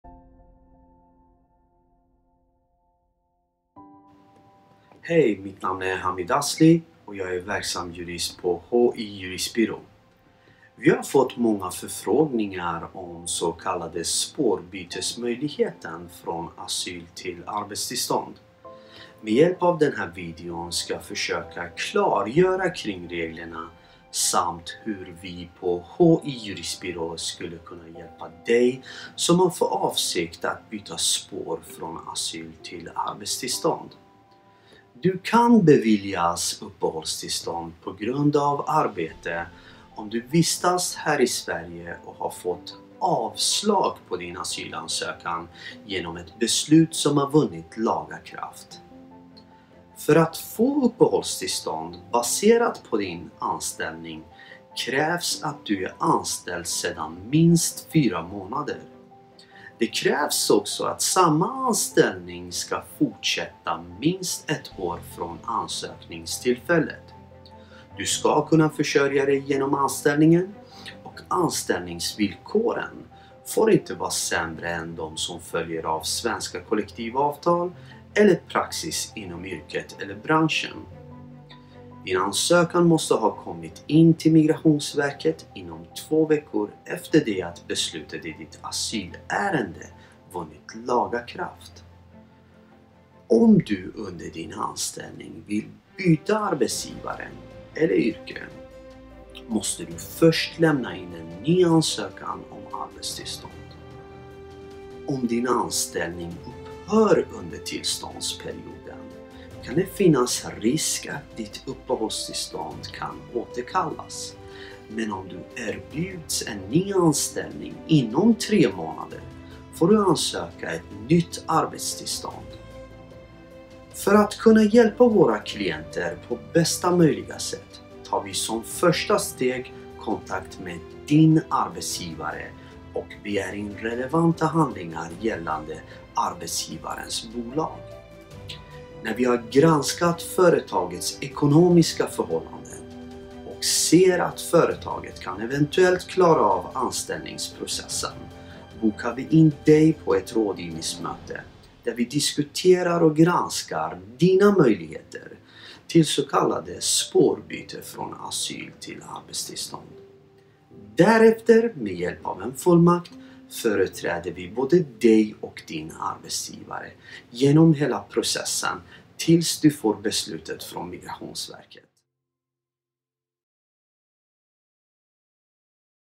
Spårbytes möjligheten från Asyl till arbetstillstånd.
Med hjälp av den här videon klargör Hamid Asli reglerna kring Spårbytes möjligheten från Asyl till arbetstillstånd samt hur vi på H I Juristbyrå skulle kunna hjälpa Dig som har för avsikt att byta spår från Asyl till arbetstillstånd.
Du kan beviljas uppehållstillstånd på grund av arbete, om du vistas här i Sverige och har fått avslag på din asylansökan genom ett beslut som har vunnit laga kraft.

För att få uppehållstillstånd baserad på din anställning krävs att du är anställd sedan minst fyra månader. Det krävs också att samma anställning ska försätta minst ett år från ansökningstillfället.

Du ska kunna försörja dig genom anställningen och anställningsvillkoren får inte vara sämre än de som följer av svenska kollektivavtal eller praxis inom yrket eller branschen.
Din ansökan måste ha kommit in till migrationsverket inom två veckor efter det att beslutet i ditt asylärende vunnit laga kraft.

Om du under din anställning vill byta arbetsgivare eller yrke, måste du först lämna in en ny ansökan om arbetstillstånd.

Om din anställning upphör under tillståndsperioden kan det finnas risk att ditt uppehållstillstånd kan återkallas.
Men skulle du erbjudas en ny anställning inom tre månader får du ansöka om ett nytt arbetstillstånd.

För att kunna hjälpa våra klienter på bästa möjliga sätt, tar vi som första steg kontakt med din arbetsgivare och begär in relevanta handlingar gällande arbetsgivarens bolag.
När vi har granskat företagets ekonomiska förhållanden och ser att företaget kan eventuellt klara av anstäpllningsprossesen, bokar vi in dig på ett rådgivningsmöte där vi diskuterar och granskar dina möjligheter till så kallad spårbyte från Asyl till arbetstillstånd.
Därefter med hjälp av en fullmakt företräder vi, både dig och din arbetsgivare genom hela processen tills du får beslutet från Migrationsverket.